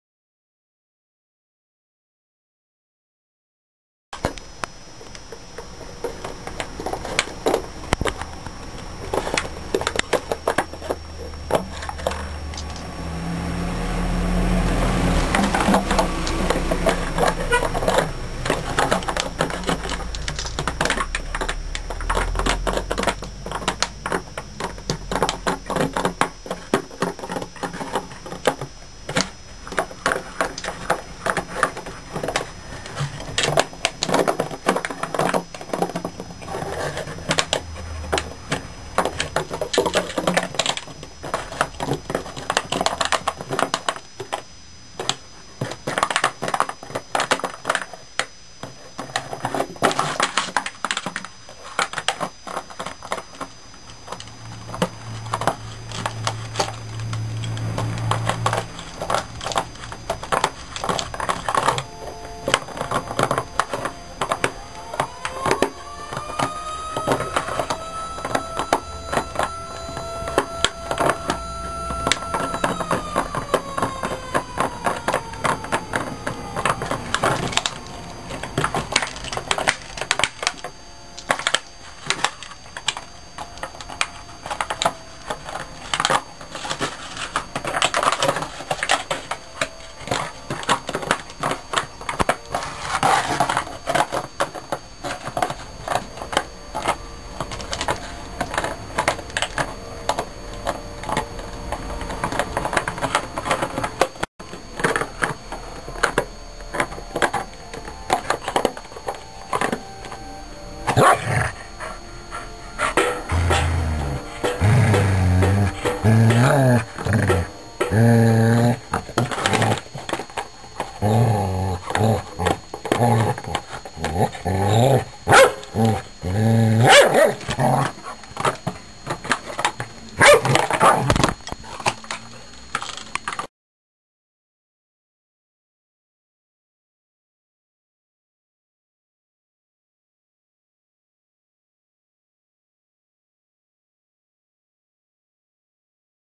The King of Destruction（破壊王「爆」）
ダルメシアンのボスはペットボトルで遊ぶのが大好き
（Dalmatian Boss likes to play with pet bottles. ）